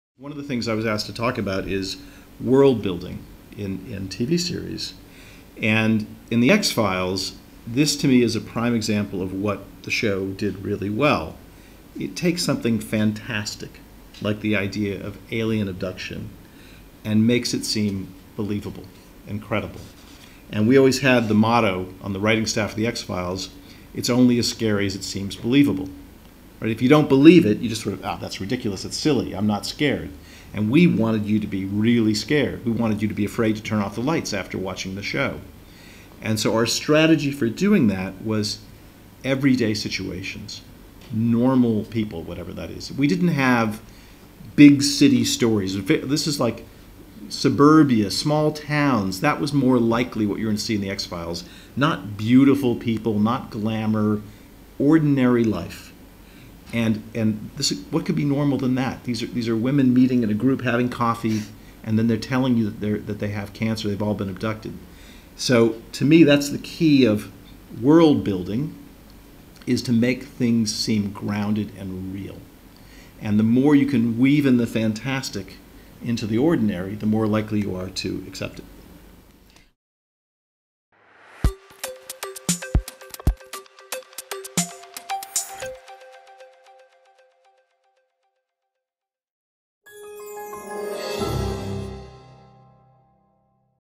RTS Masterclass with Frank Spotnitz - Ep. 3
As part of a masterclass at the Royal Television Society, hosted by Caroline Whitmore, Frank Spotnitz elaborates on the tasks of becoming a professional writer, particularly about "World Building" on @THEXFILESFOX.
Copyright: Royal Television Society
Authorized use only for Big Light Productions associated content channels.